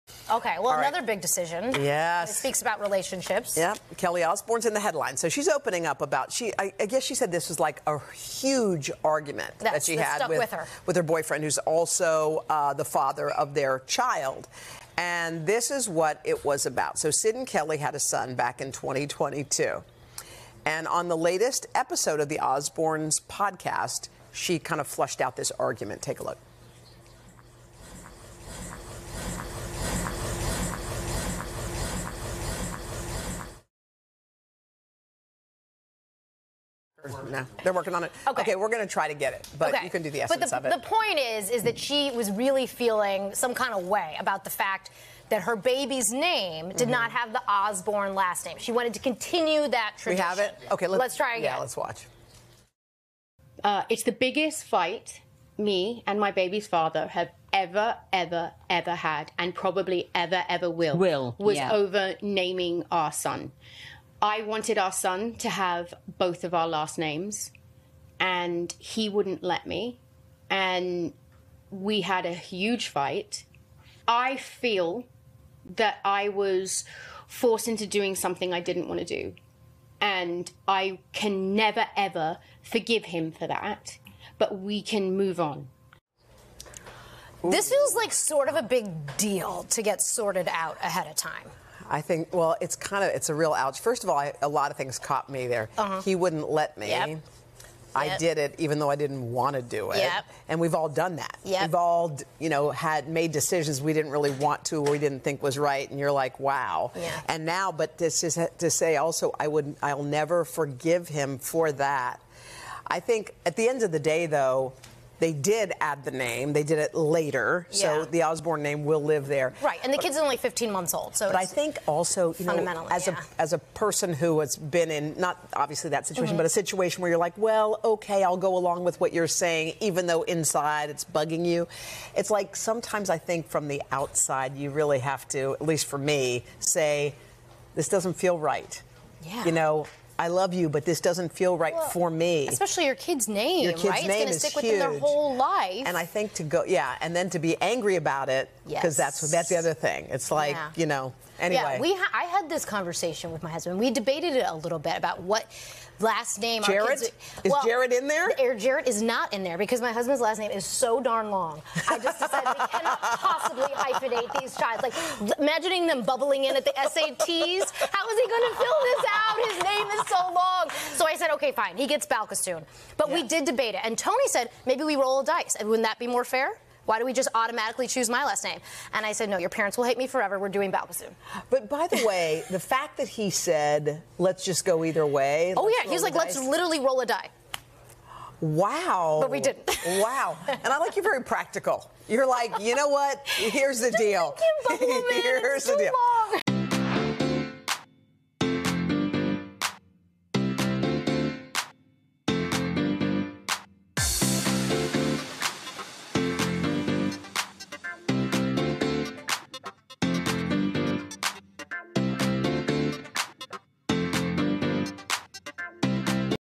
Kelly Osbourne gets candid about argument over baby's last name
Kelly Osbourne recently opened up on "The Osbourne Podcast" about an argument she had with the father of her baby over the last name they'd give their son. "I felt I was forced into something I didn't want to do," she says. Hoda Kotb and guest co-host Laura Jarrett weigh in.